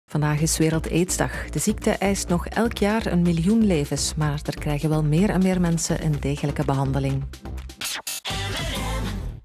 aidsdag
Vandaag is Wereldaidsdag. De ziekte eist nog elk jaar meer dan een miljoen levens. Er krijgen wel meer en meer aidspatiënten een degelijke medische behandeling. Dat komt omdat de medicatie toegankelijker is geworden.